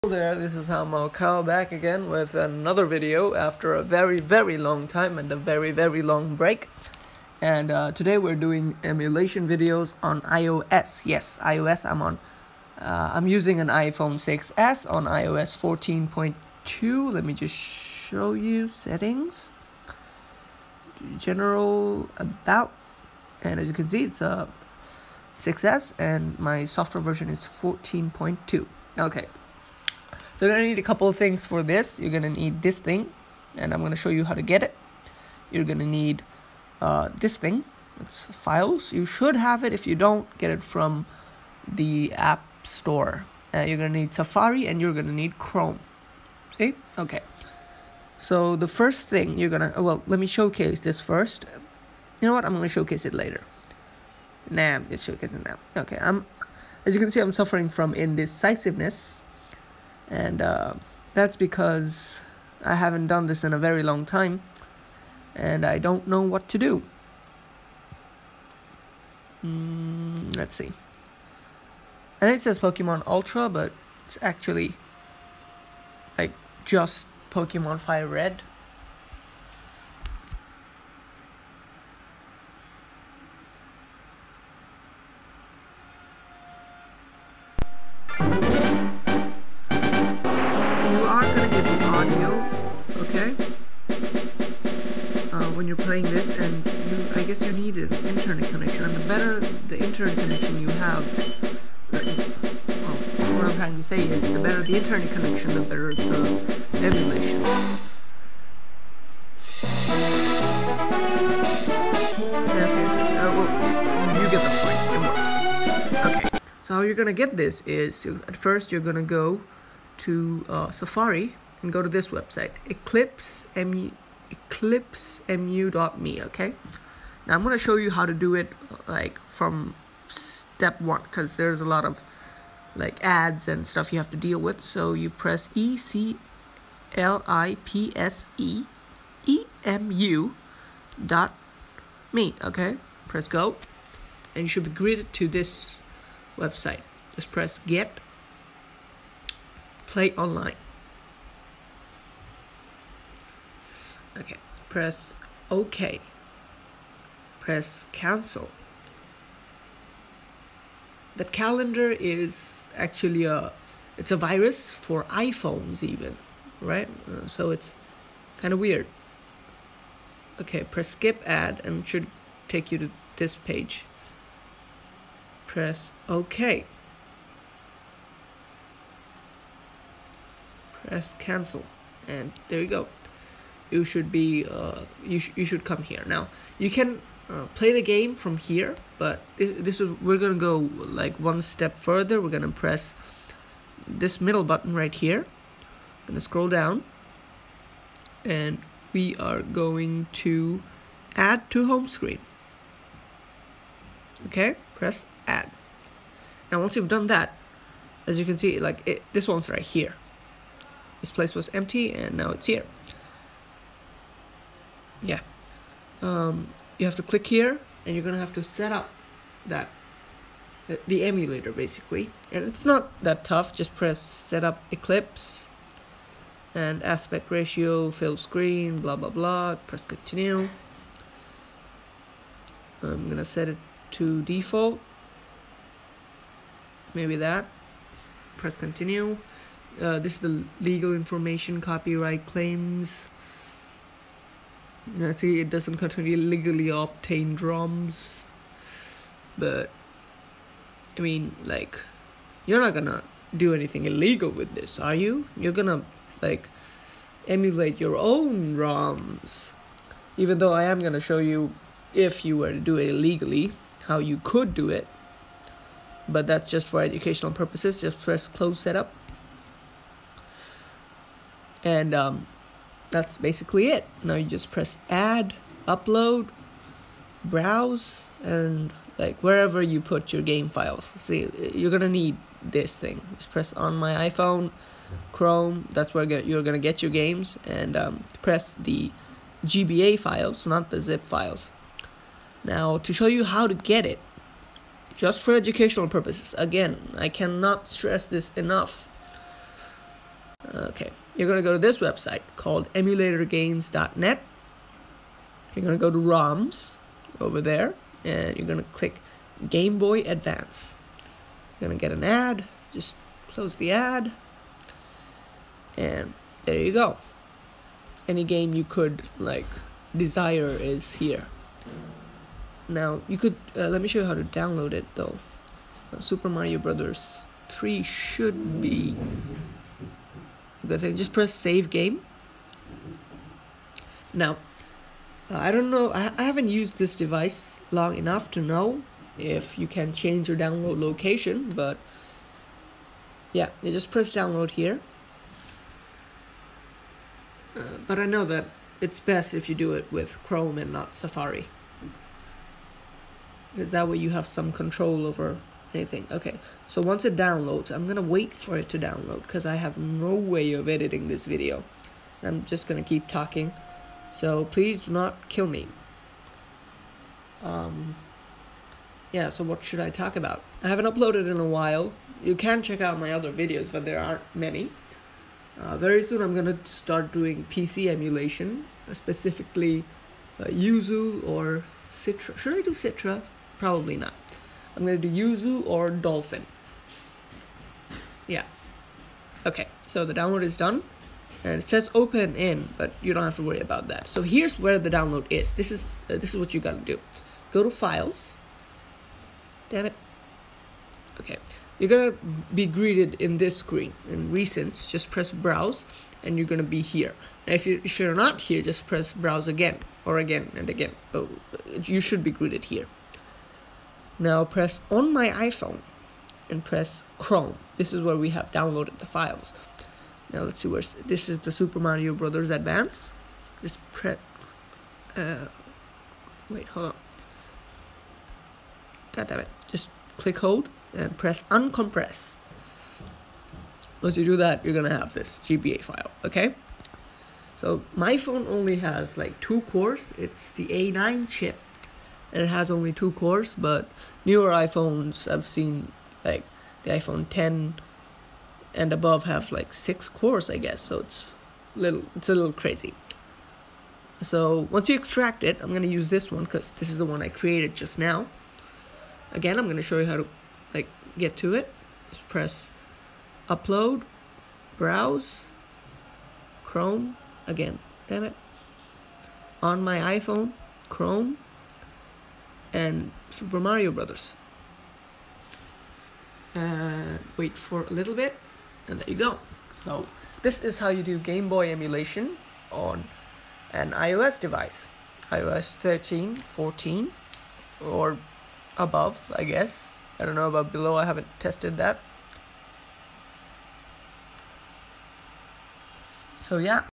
How to emulate GBA on any iPhone/ipad | No jailbreak | No Revoke | No computer!(iOS 12, 13, 14)
This is a GBA emulation tutorial for iphones using eclipse emulator . I am using the iphone 6s and it should work on almost any iphone.

You can save your progress through the game normally and through the emulator itself.

It is a browser based emulator so there is no chance of getting revoked.

eclipseemu.me

Just do a google search.

(Comment if you need help)

Good luck